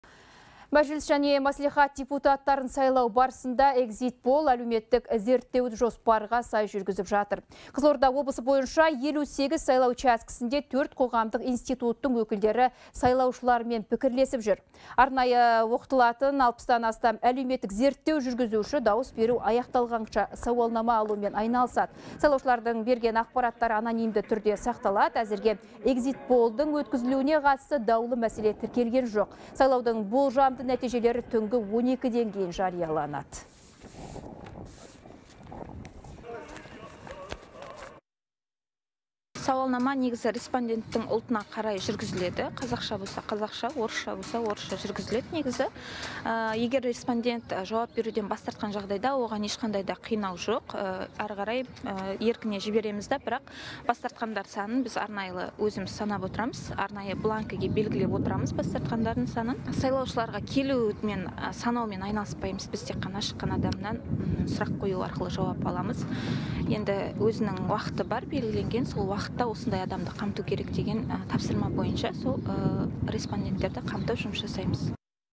Қызылордада exit poll бойынша 60 адам зерттеу жүргізіп жүр
Мәжіліс және мәслихат депутаттарын сайлау барысында «Экзит пол» әлеуметтік зерттеуі жоспарға сай жүргізіліп жатыр.